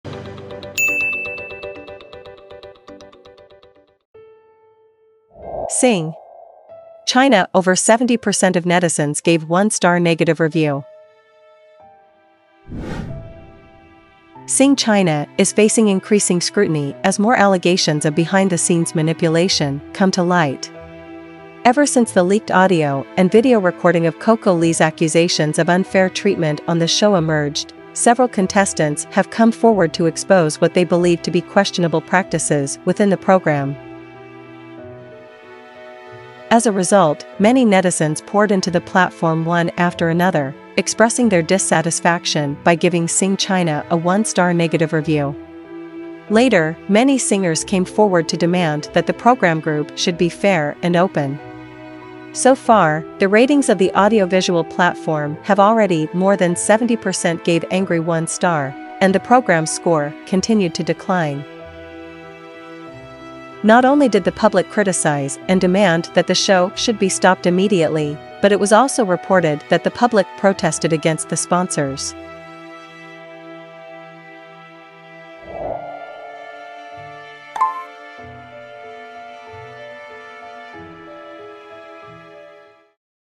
(News) "Sing! China" Over 70% Of Netizens Gave "1 Star Negative Review"
"Sing! China" is facing increasing scrutiny as more allegations of behind-the-scenes manipulation come to light. Ever since the leaked audio and video recording of CoCo Lee’s accusations of unfair treatment on the show emerged, several contestants have come forward to expose what they believe to be questionable practices within the program.